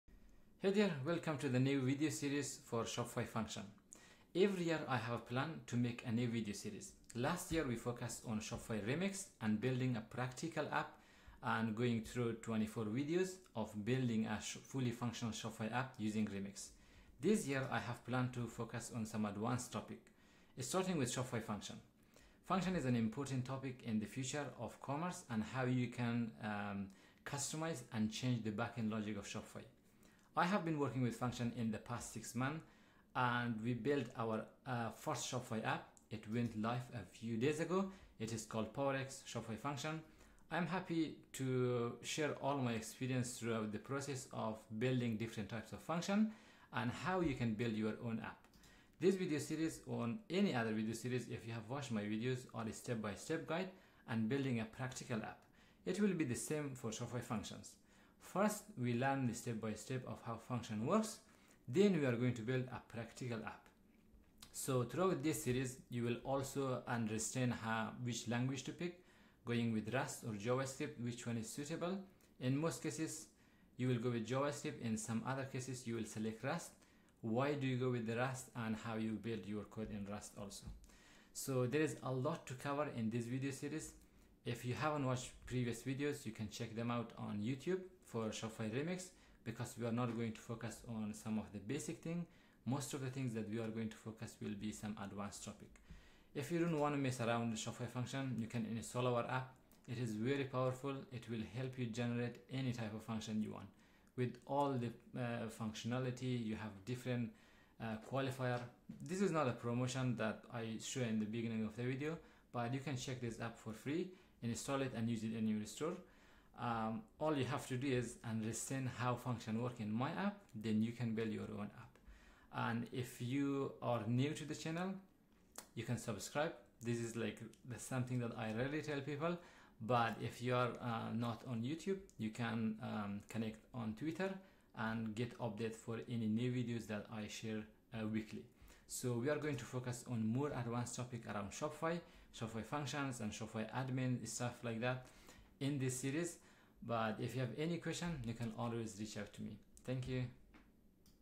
Shopify Functions - Practical Course
Hi there.

Welcome to the new video series for Shopify Functions. This year, I have plan to focus on more advanced topics of Shopify. Starting with Functions and how to build apps that changes Shopify back-end logics.

If you want to check out Function generator app, Here is the link to try it for free